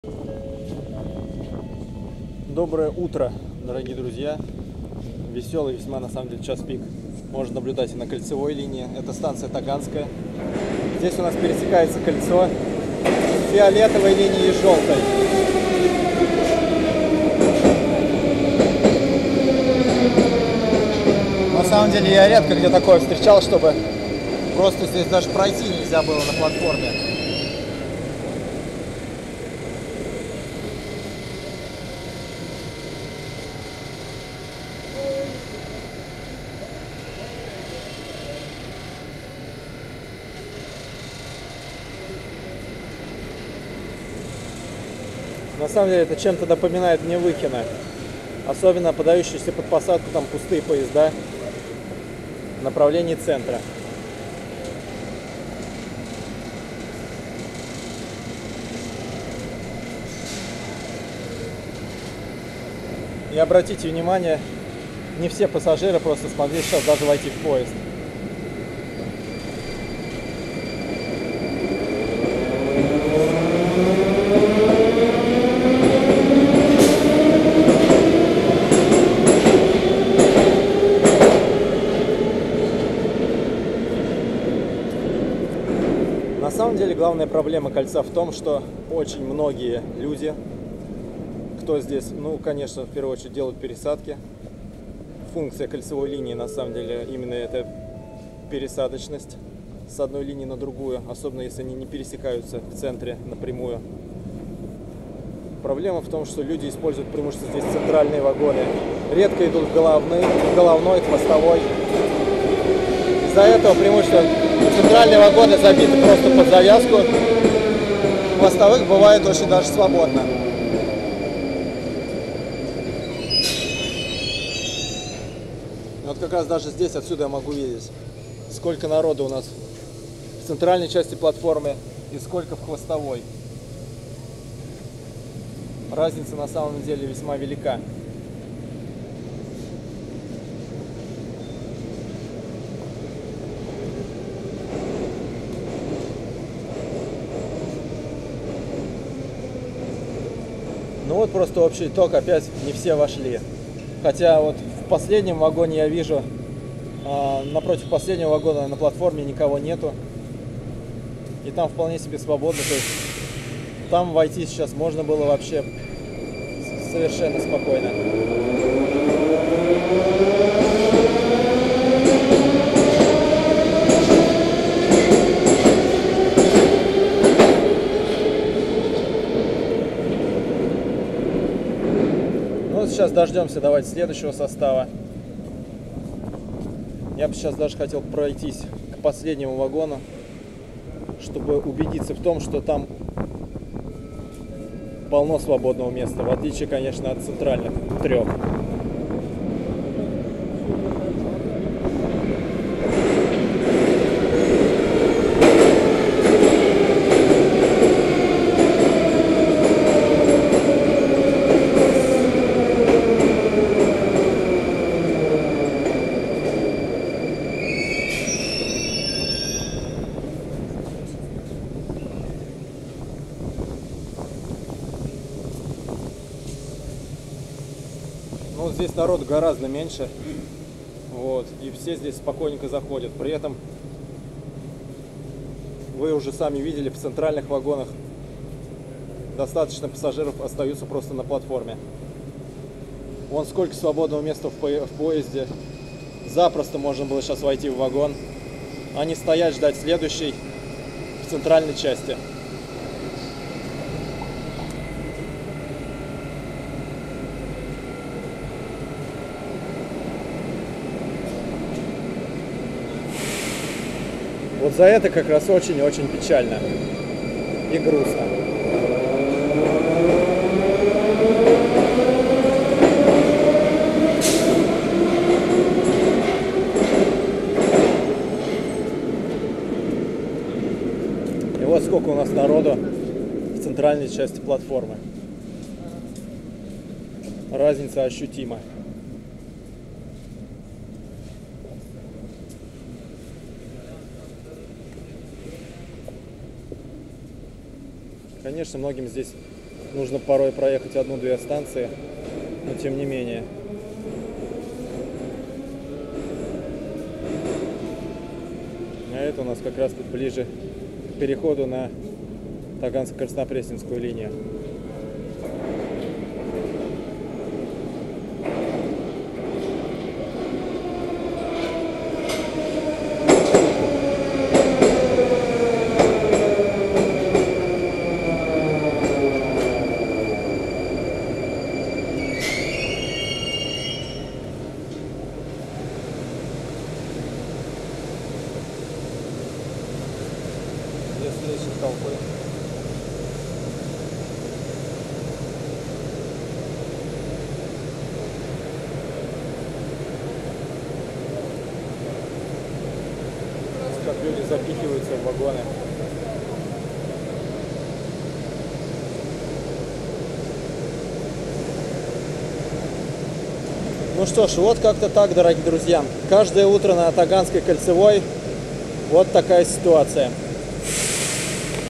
Часы-пик на транспорте - метро "Таганская" (кольцевая)
Утренний "час-пик" на станции метро "Таганская" Кольцевой линии. Ситуация напоминается станции на южном участке Таганско-Краснопресненской линии - у пассажиров порой не удается зайти в поезд с первой попытки. Мало где вне Таганско-Краснопресненской линии можно встретить такие очереди...